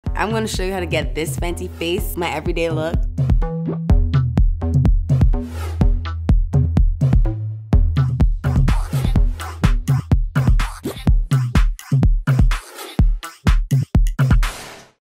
How to achieve Rihanna’s everyday look aka ✨THE FENTYFACE✨ peep the description for the steps!! 👀
Fenty Fam, we're about to let you in on some juicy ishhh 👀 Follow along to peep how to achieve Rihanna's everyday look aka ✨THE FENTYFACE✨

1️⃣ CHOOSE YOUR BASE
Start with the coverage of your choice, PROFILTRFOUNDATION for full coverage or EAZEDROP Skin Tint for lighter coverage 👏🏽

2️⃣ SHAPE YOUR FACE
Show off the best features ya mama gave ya with MATCHSTIX Contour Skinstick 😋

3️⃣ ADD HIGHLIGHT WHERE YOU WANT
Take it up a notch and get hella natural or hella lit with KILLAWATT 🌟 Muva uses shades 'Girl Next Door/Chic Phreak'

4️⃣  TOUCH UP & SET
Touch up with PROFILTR Powder Foundation or blot and set with INVISIMATTE to keep your makeup lookin fresh af!

↪️ THE FINISHING TOUCH
Y'all thought we were done? You know we can't leave home without a swipe of GLOSSBOMB ‼️